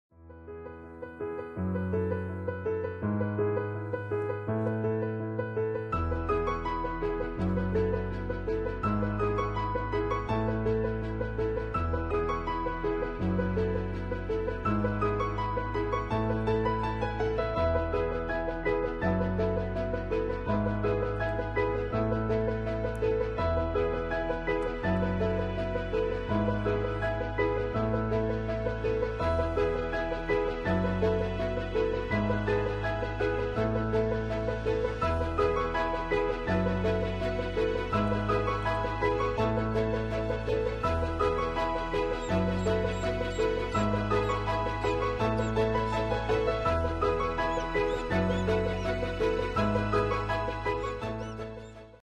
فراخ البط hatch les oeufs de canard et sortir les poussi
كيفية انعاش صغار البط بعد خروجها من البيضة قبل وقتها بيوم واحد